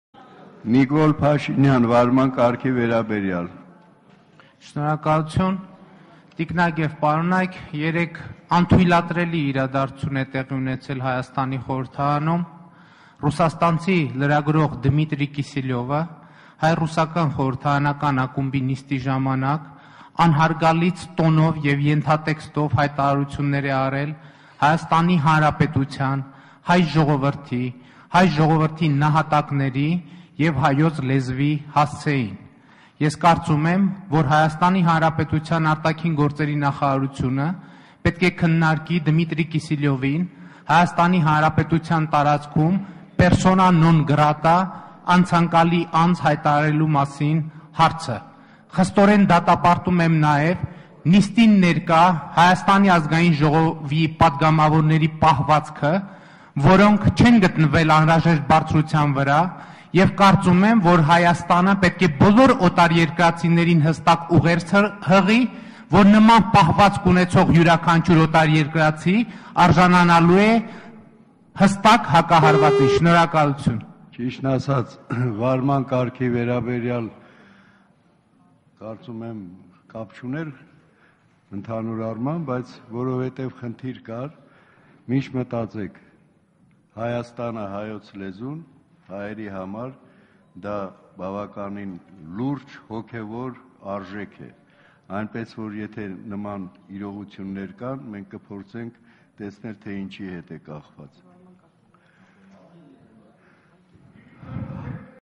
Անթույլատրելի իրադարձություն է տեղի ունեցել Հայաստանի խորհրդարանում. Նիկոլ Փաշինյան
Nikol Pashinyan, Galust Sahakyan